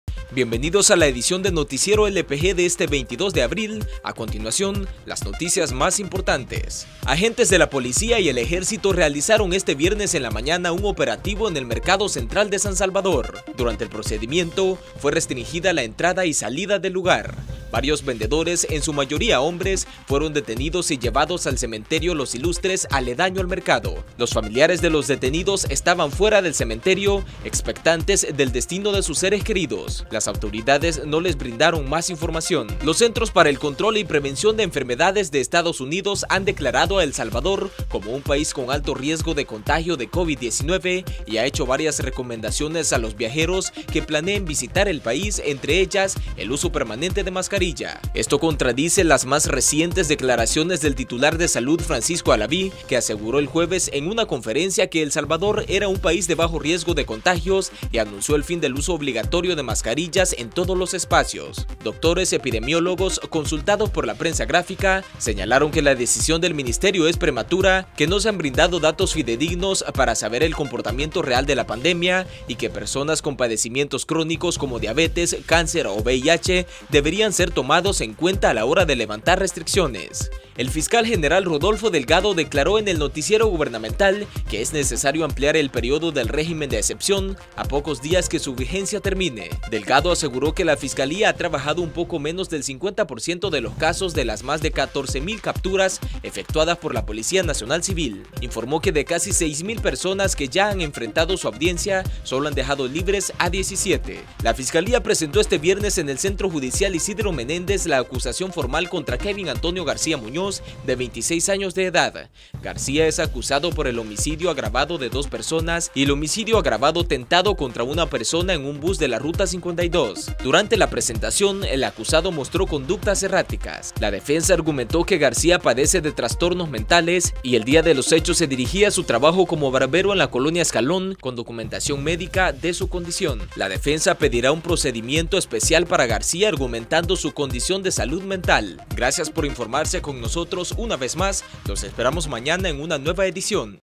Noticiero LPG 22 de abril: Ejecutan operativo en mercado Central de San Salvador
Agentes de la Policía y el Ejército realizaron este viernes en la mañana un operativo en el Mercado Central de San Salvador. Durante el procedimiento fue restringida la entrada y salida del lugar.

¡Visítenos!